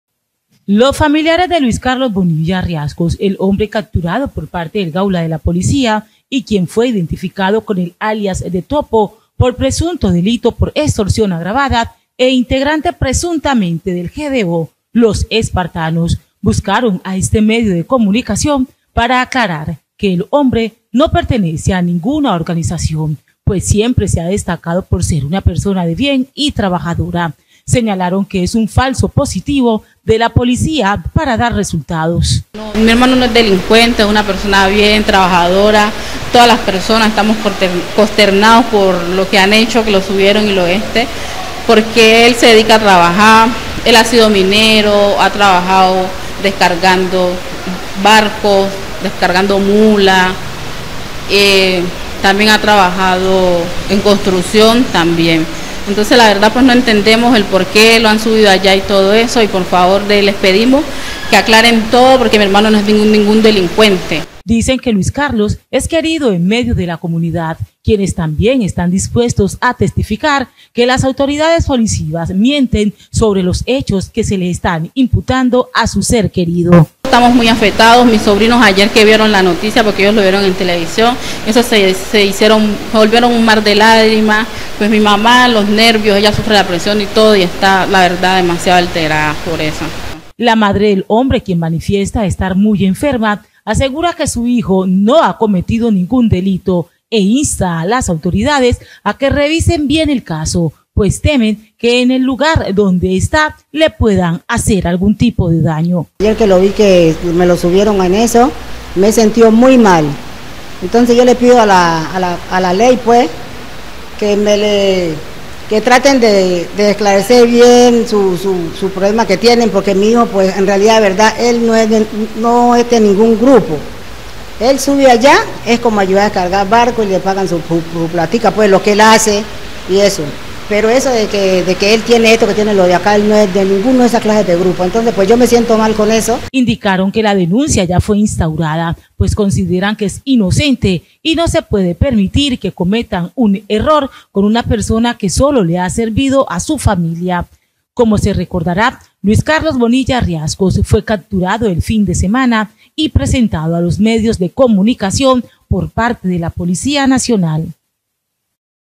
DETENIDO Y FAMILIA
Las Autoridades dieron captura a un hombre con el alias de “topo”, en días pasados sindicado por diferentes delitos, hoy los familiares de Luis Carlos Bonilla Riascos manifiestan que es totalmente falso y se trataría de un falso positivo.